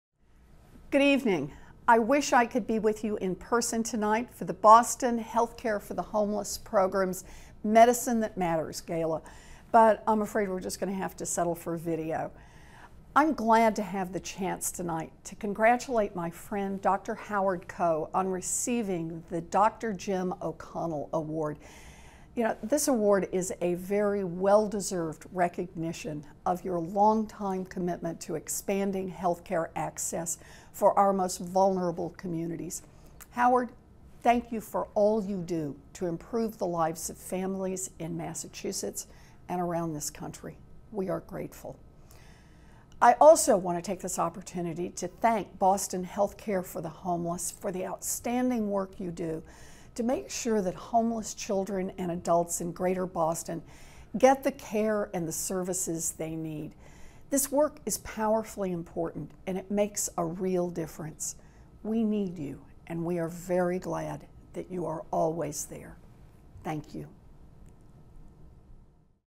Medicine That Matters Gala 2015: Message from Elizabeth Warren
Senator Elizabeth Warren congratulates Dr. Howard Koh on receiving the Dr. Jim O'Connell Award at our 2015 Medicine That Matters Gala.